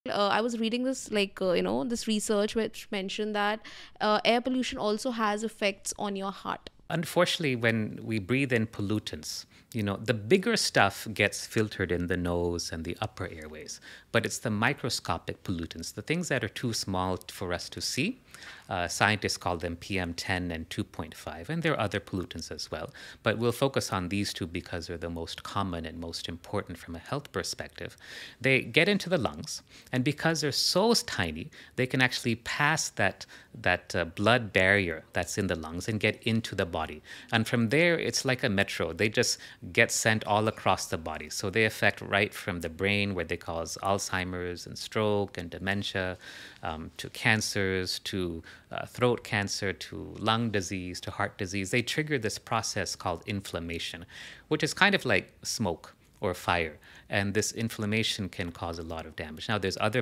Air pollution a cause for increasing Alzheimer's and heart attacks in India
⚠️Cardiologist answers how air pollution causes Alzheimer’s, Stroke, Dementia, Throat Cancer, Heart Attacks!   Pollution free India

Dr. Srikanth Sola @devicearth